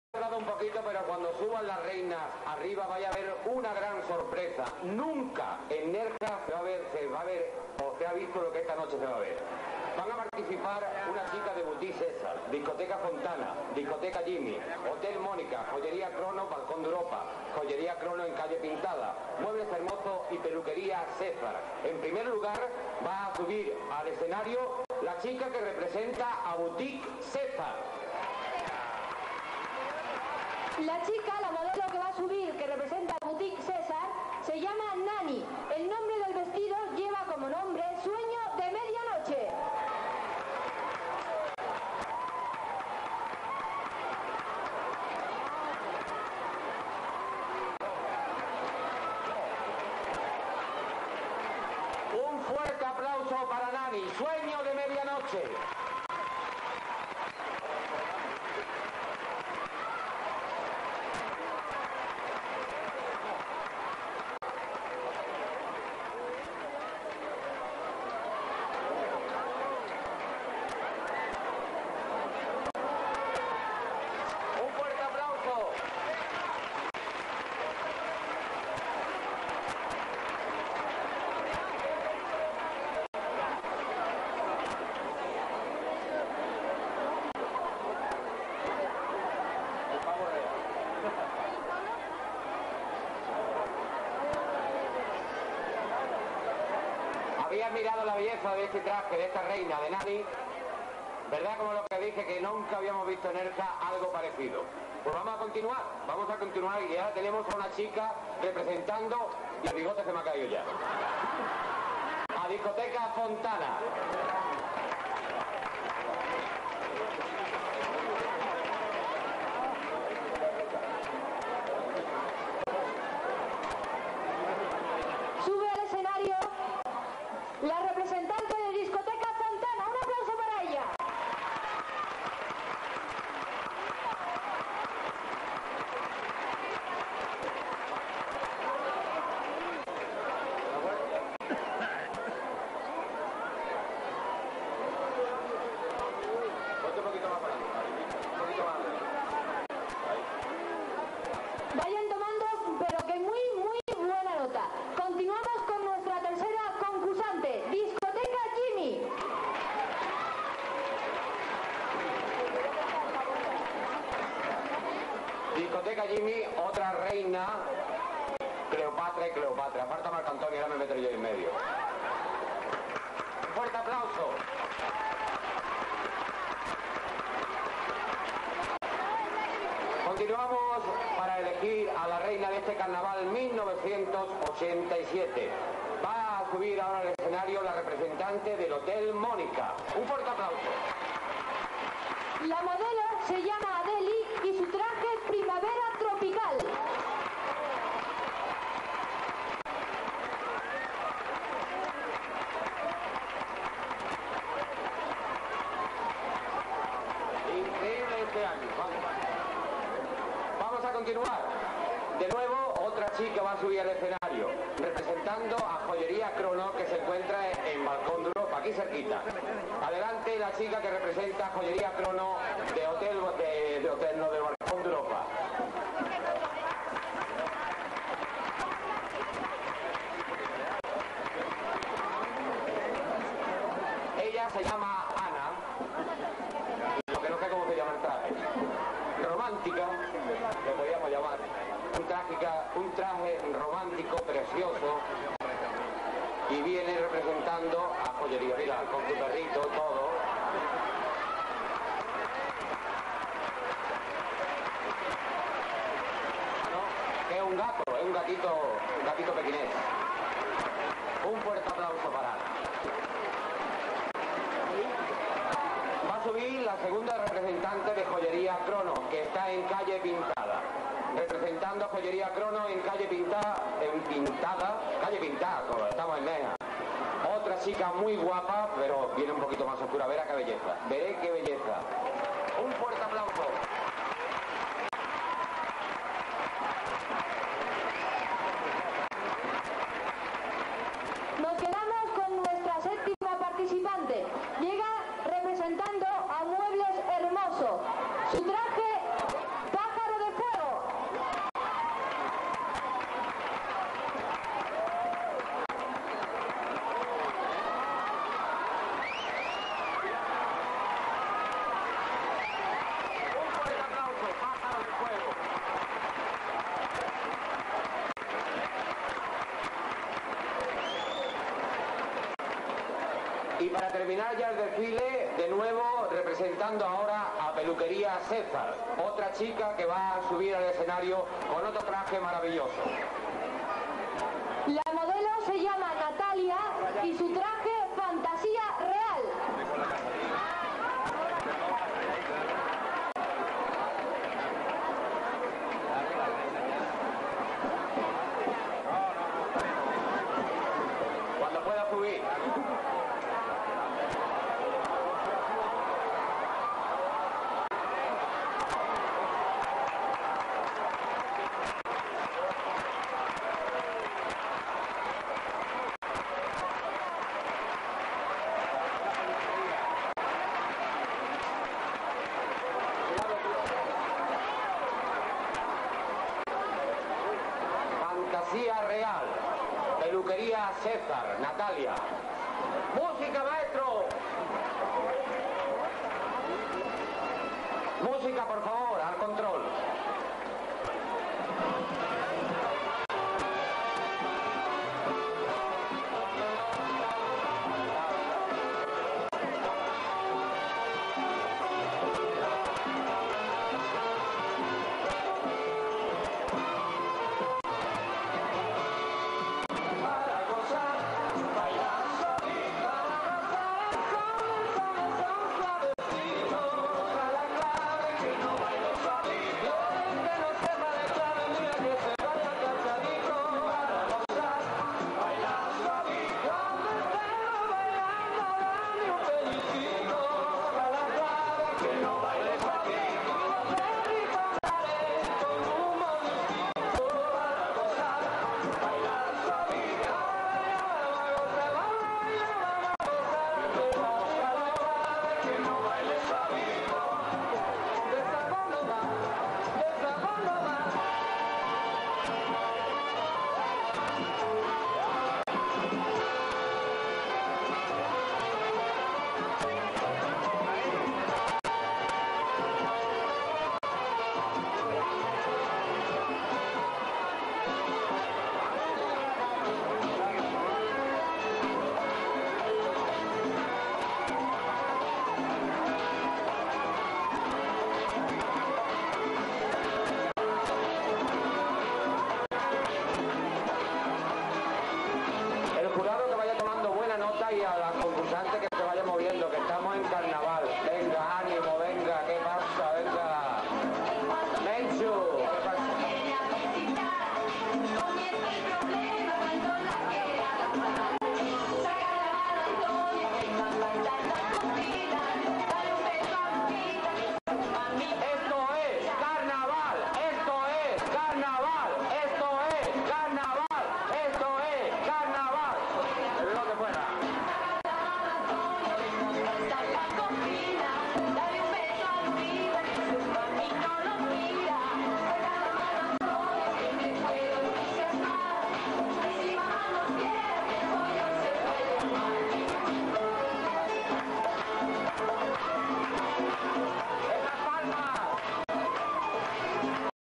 NERJA - CARNAVAL 1987 - ELECCIÓN REINA DEL CARNAVAL parte 1
Candidatas a Reina del Carnaval 1987 de Nerja